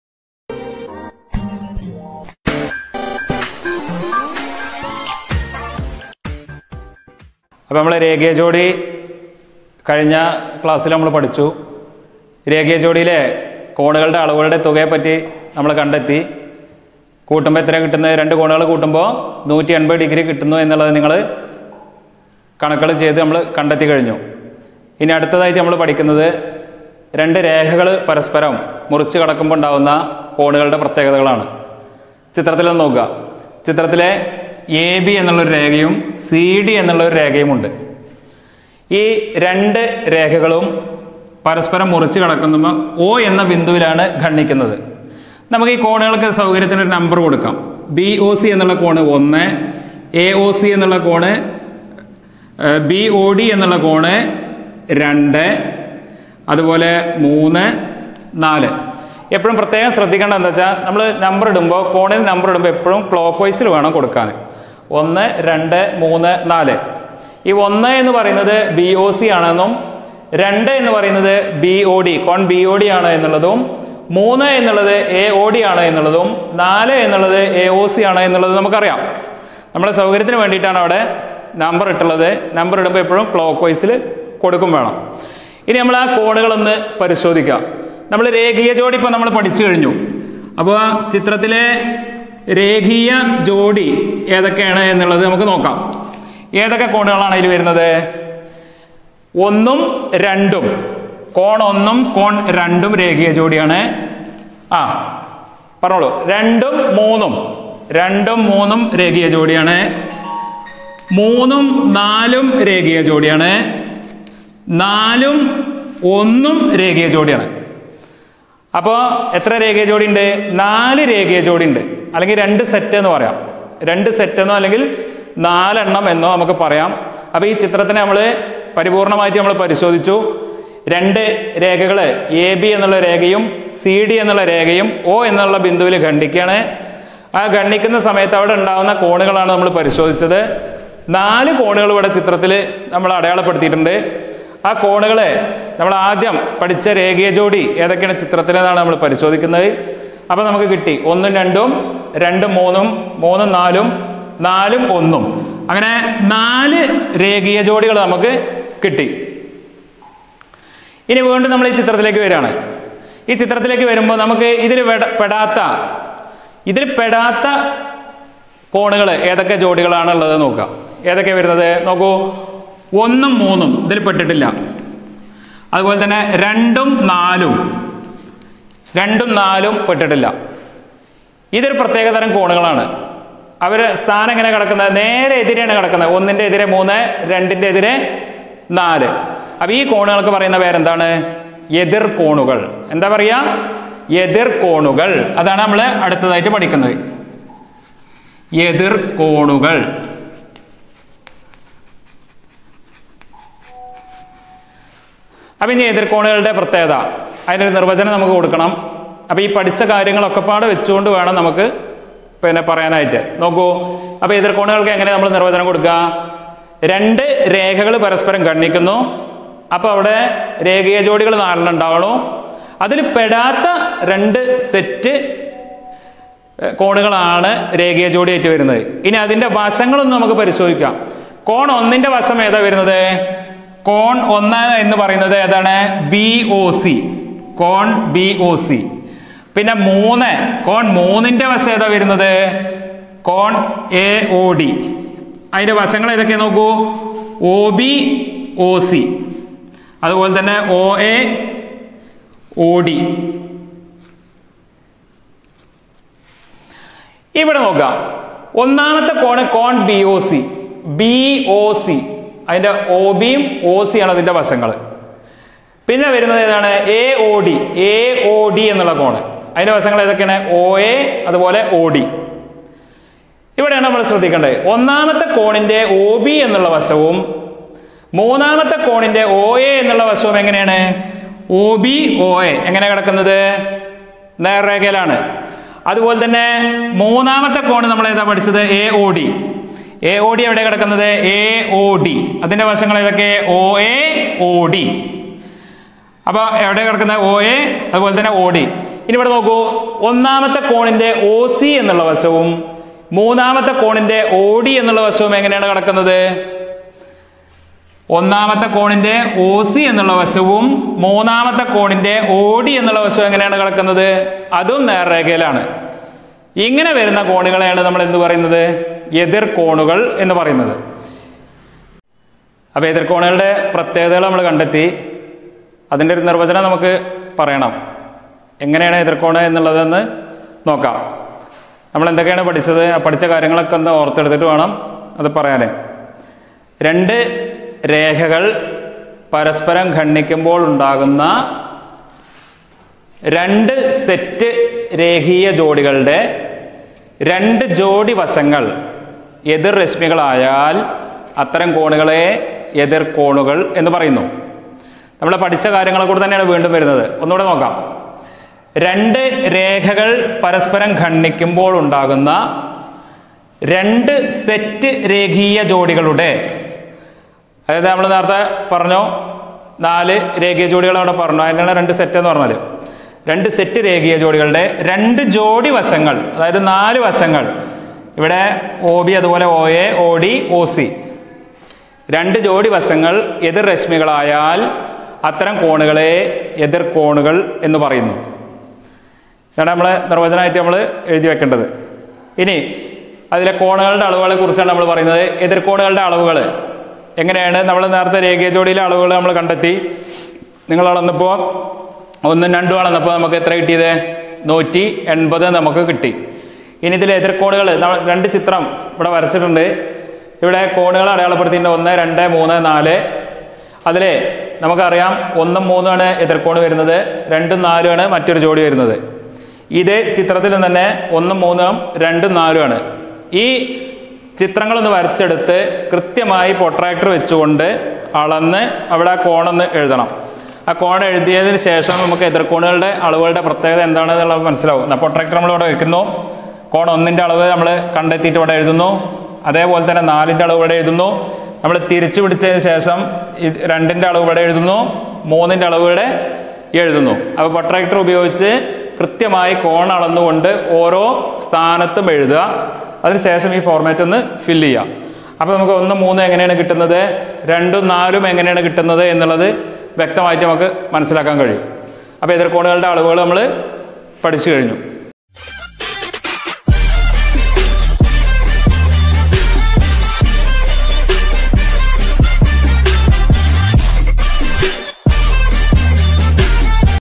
CLASS:7 || MATHEMATICS || യൂണിറ്റ്:1-കോണുകൾ ചേരുമ്പോൾ (ഭാഗം 3-എതിർ കോണുകൾ)
ക്ലാസ് അവതരണം:മുരളി മാസ്‌റ്റർ